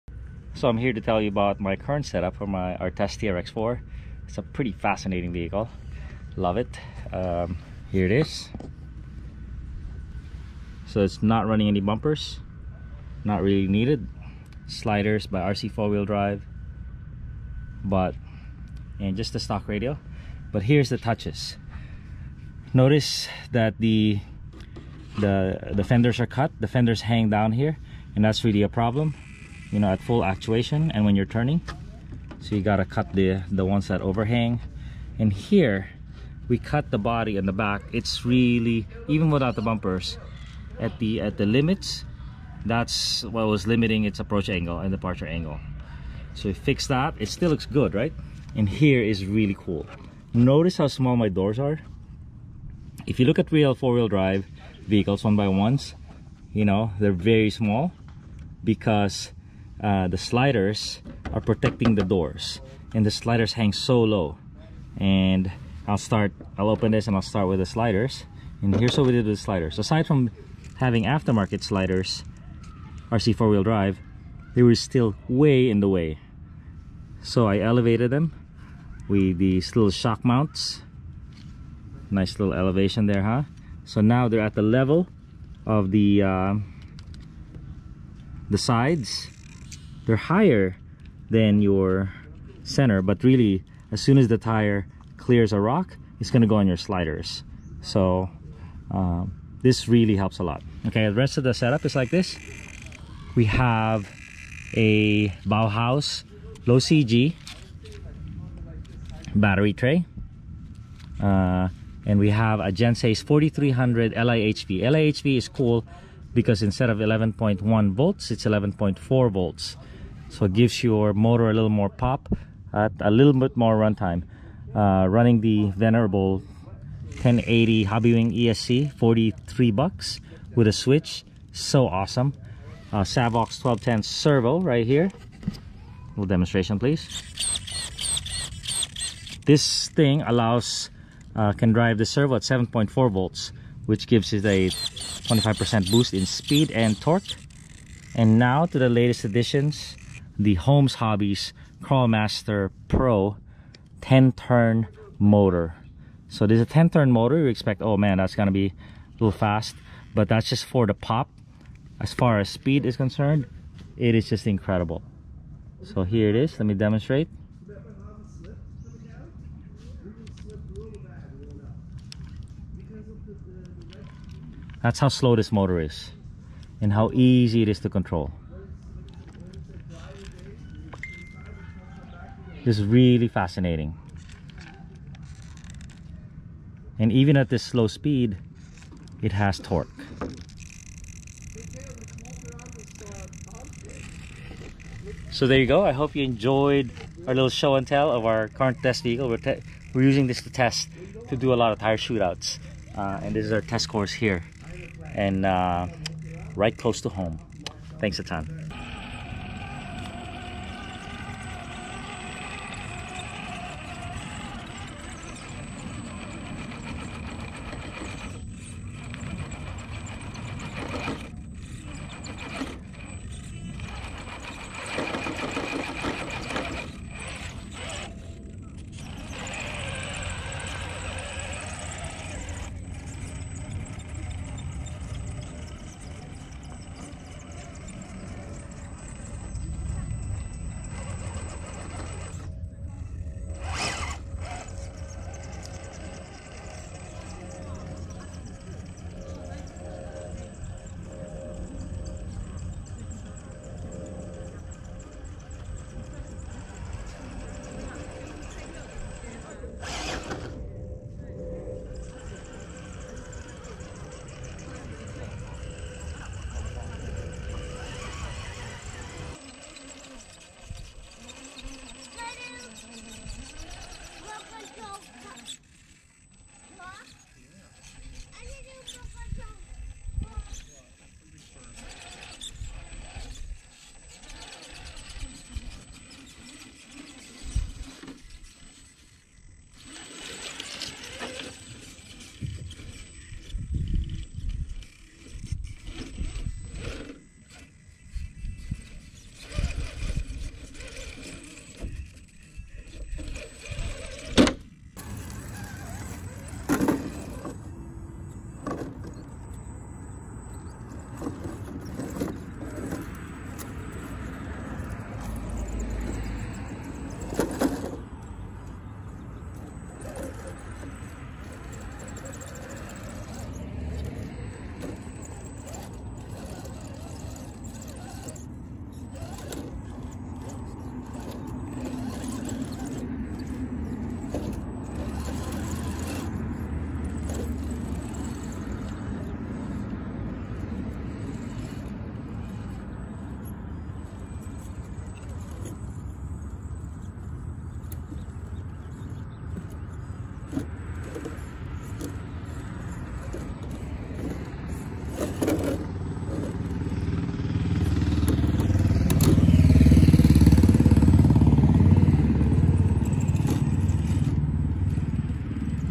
Traxxas TRX-4 Sport Best Upgrades and Mods
We go over our latest upgrades on our TRX-4 Sport and test them rc crawling on the rocks and trail. Check here for specs and prices:

Buy your kits and upgrade parts from this Amainhobbies link to support RC Review

for early videos and extra footage.